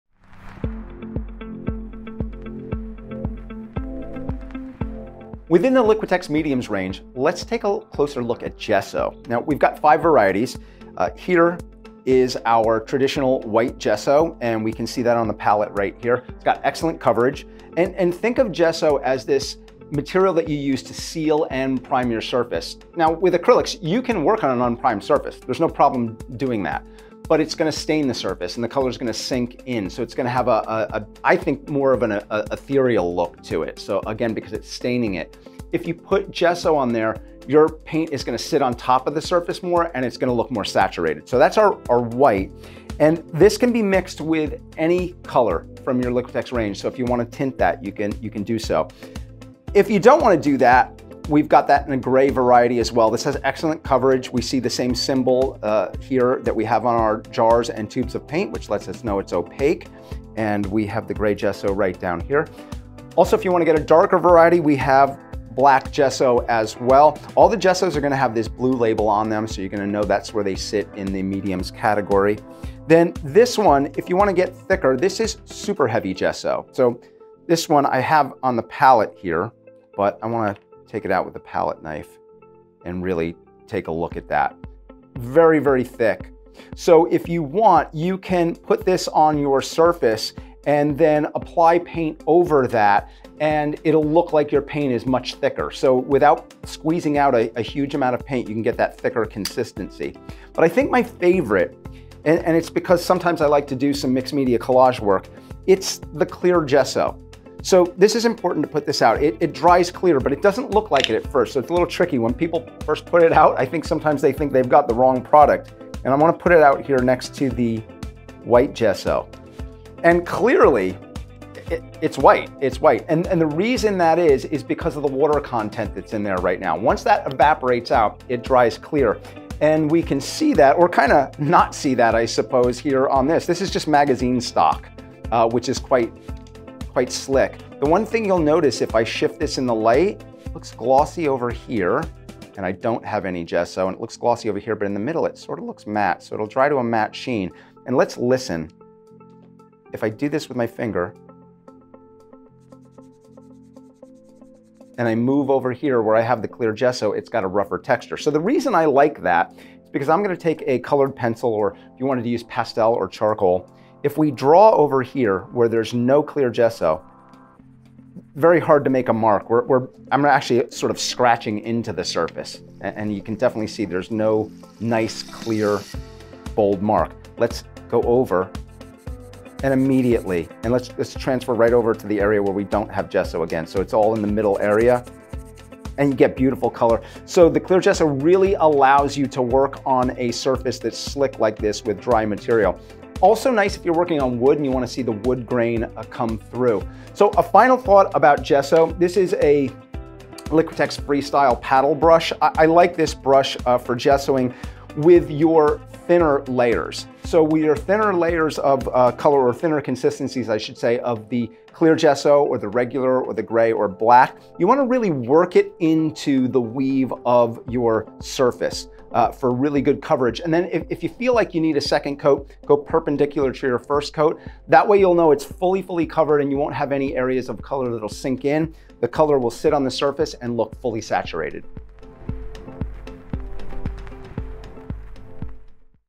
A Closer Look At The Liquitex Pro Gesso Range | Acrylic Painting
Liquitex resident artist Jimmy Leslie shows us through all 5 of the Liquitex Pro gessoes - from gray gesso to gesso that securely locks 3D texture in place. Find out how each looks on the canvas, its uses, effects and consistency as Jimmy gives inside tips on quick studio hacks and what to avoid.